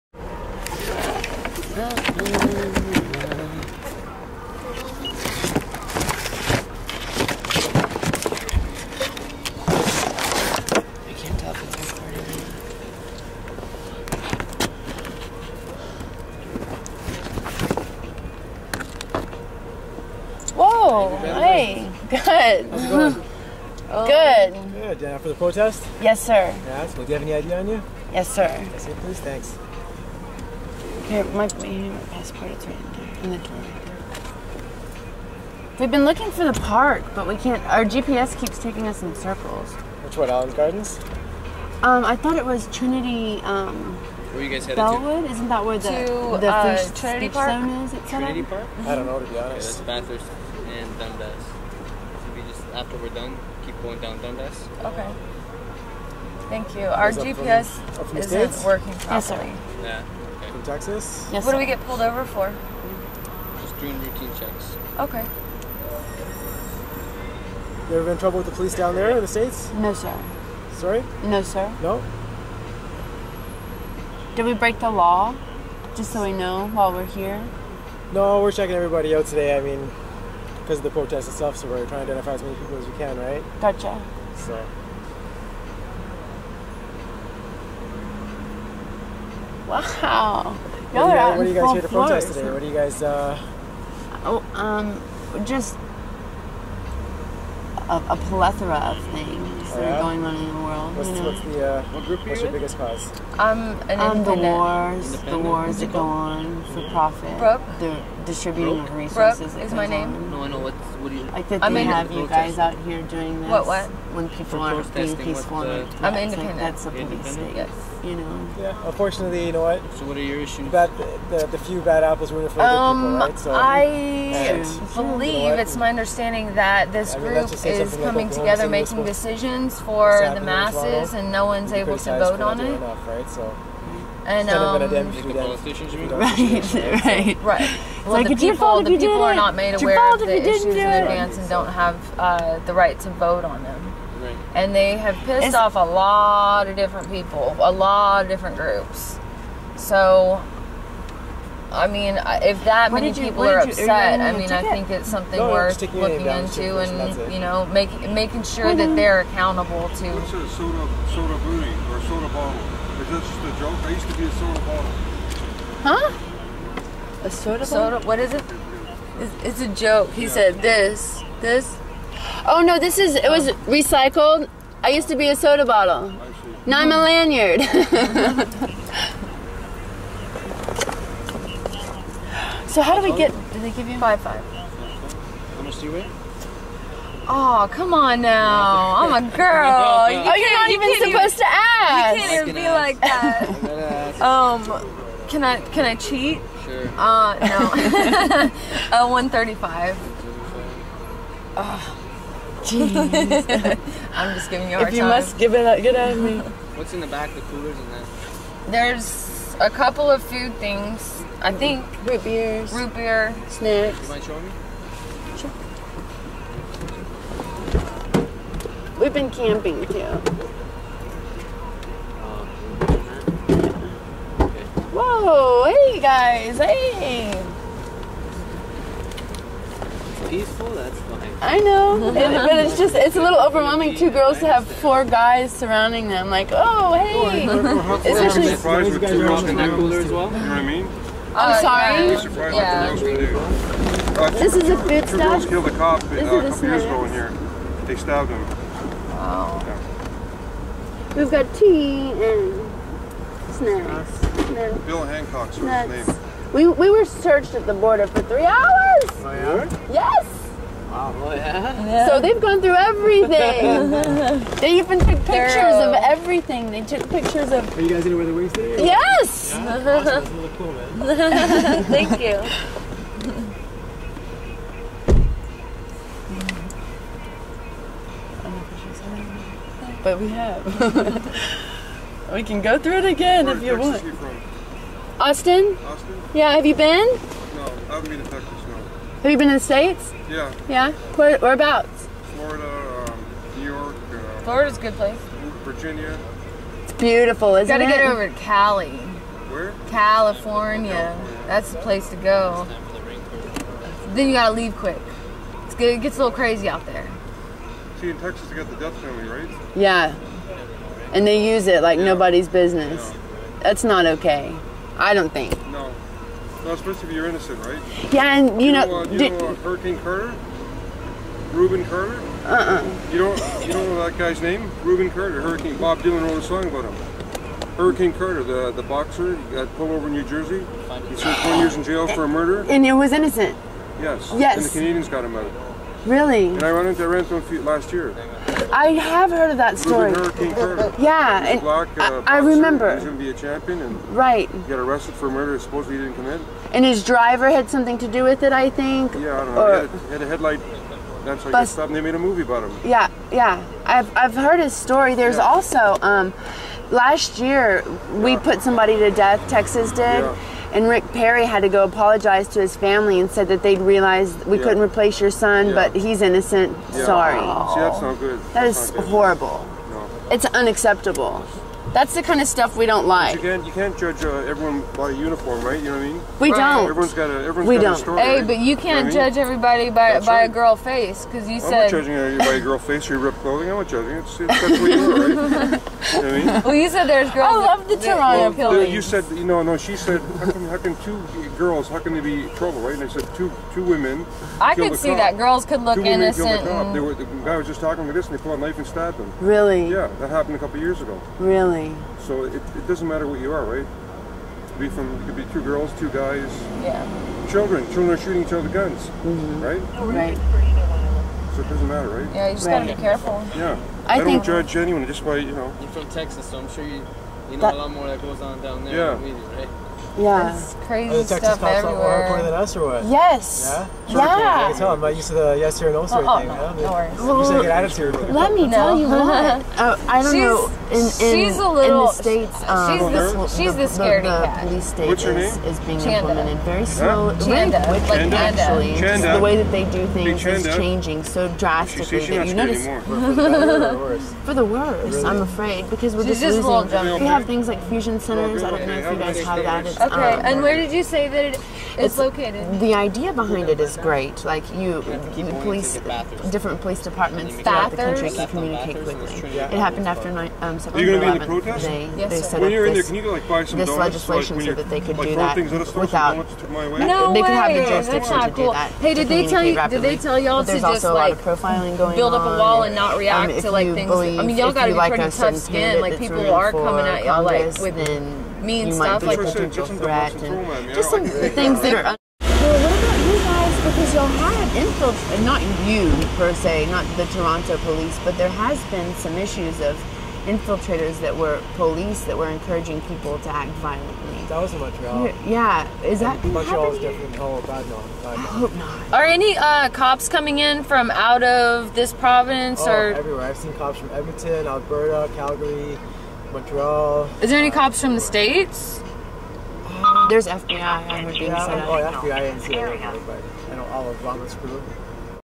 Toronto G20 Summit, Random Check
Brooke and Chanda interview the Toronto Police!